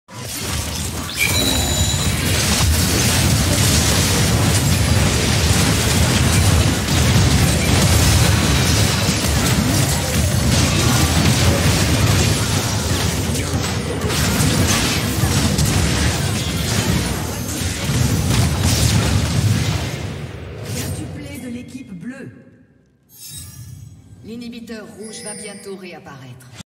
Penta avec mon main 💥💥🔥🔥
Mon premier penta de la saison avec mon main. C'est pas ouf mais je suis assez content 💥